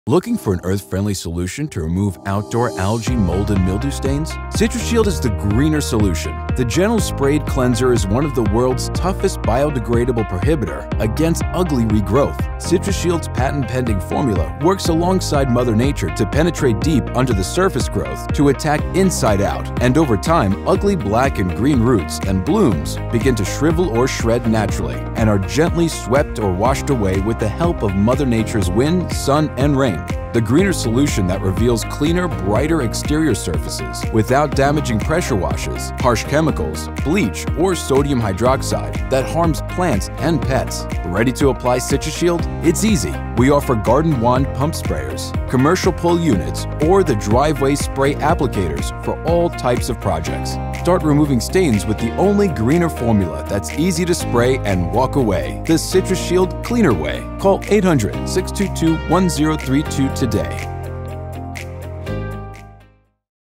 Citra-Shield: Mold, Algae, Mildew Cleaner & Preventative
Keep your exterior surfaces free of algae, mildew, mold and fungus with Citra-Shield Cleaning Systems. Our eco-friendly cleaning products are the solution to all of your exterior cleaning needs. With the help of Mother Nature, Citra-Shield gently cleans the treated surface and prevents algae from growing back. Our products do NOT contain bleach, acids, phosphates or lyes.